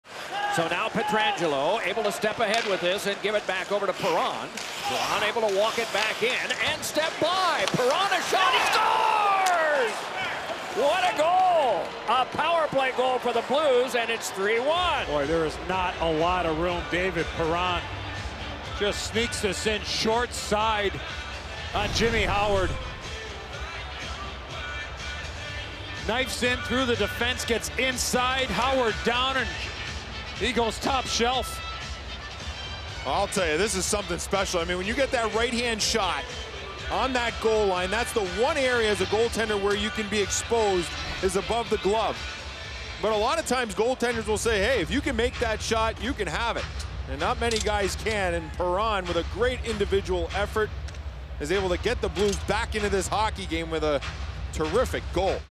David Perron splits Red Wings defenders, finishes top shelf
St. Louis Blues forward David Perron stickhandles between a pair of Red Wings defenders and quickly roofs a shot on the short side of Jimmy Howard for a terrific goal

Feeling social?